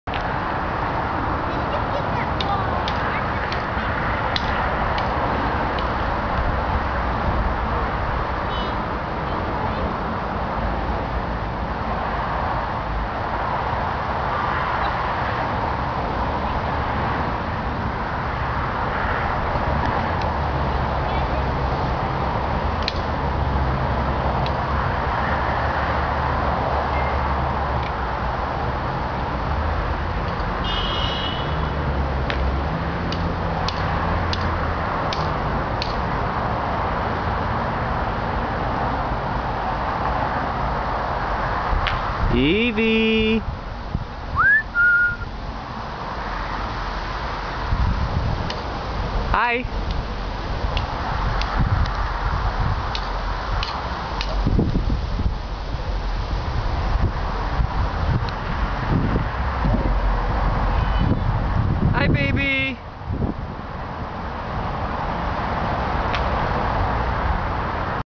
First game of Catch with Evie
Outside in the backyard enjoying a nice game of catch. Well, it’s more like fetch right now (you have to start somewhere).

Magee Family Video
editor: j.g.magee
lead: evie